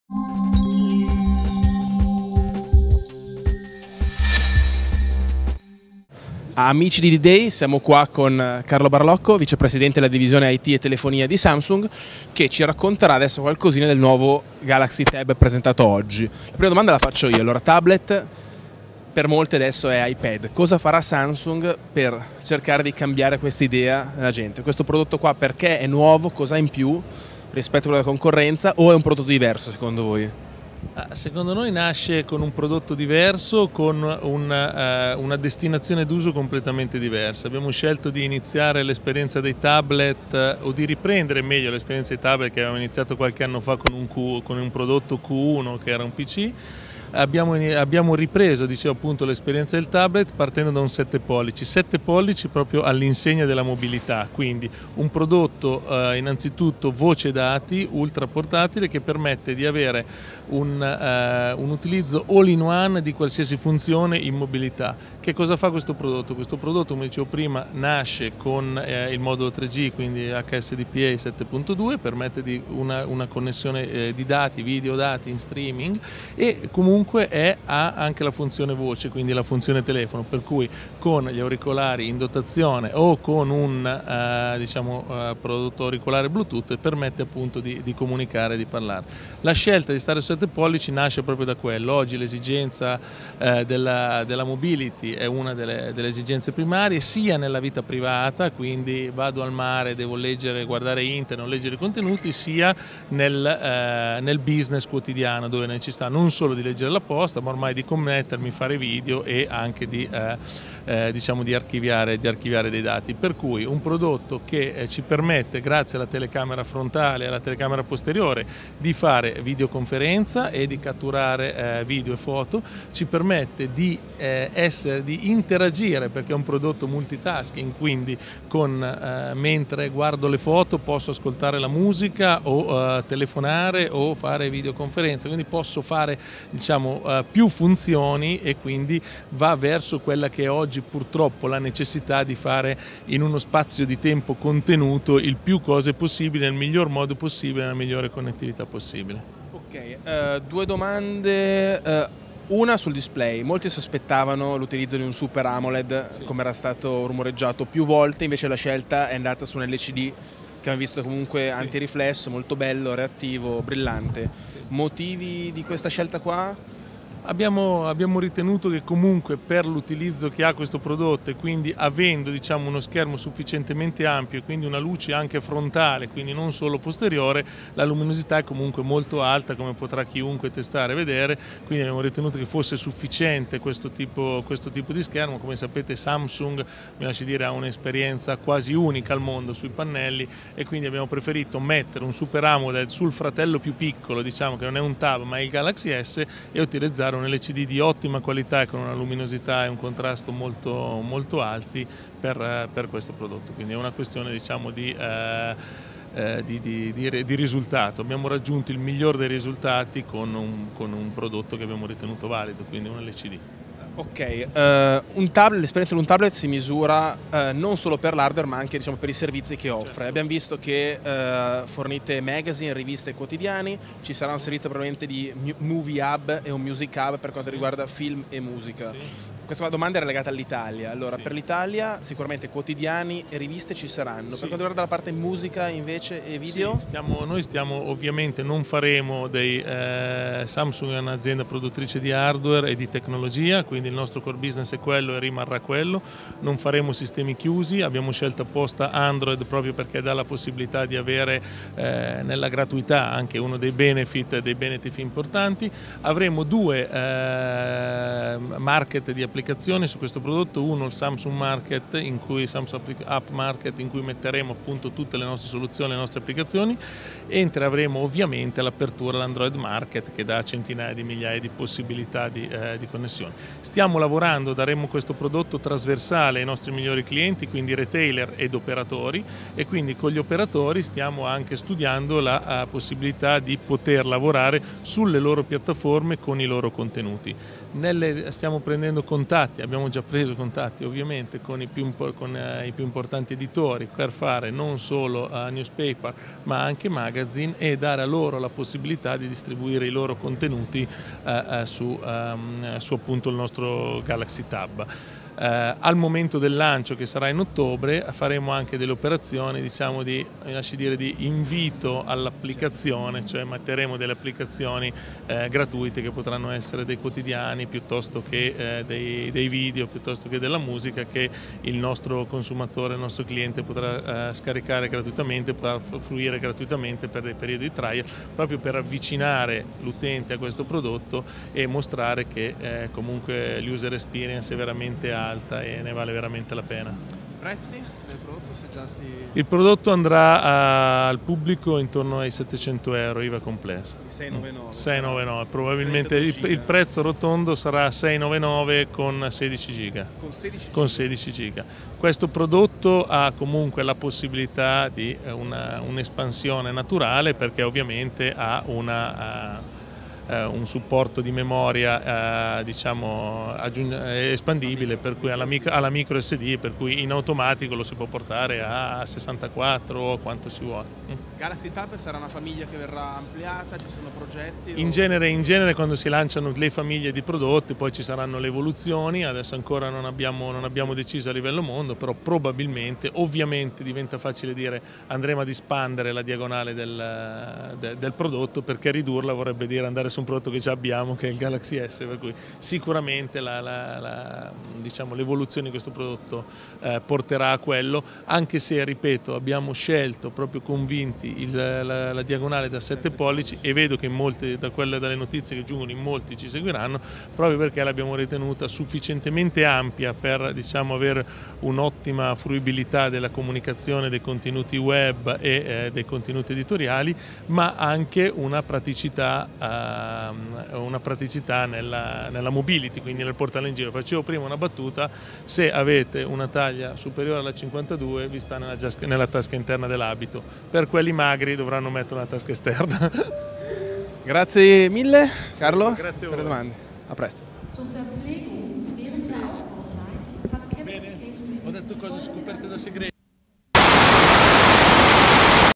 Samsung Galaxy Tab raccontato del Vice President di Samsung, Carlo Barlocco
Una veloce intervista a Carlo Barlocco di Samsung per farci raccontare tutto quello che non è stato detto nella presentazione ufficiale del Galaxy Tab: perchè manca l'OLED? Che servizi saranno attivi in Italia. E il prezzo?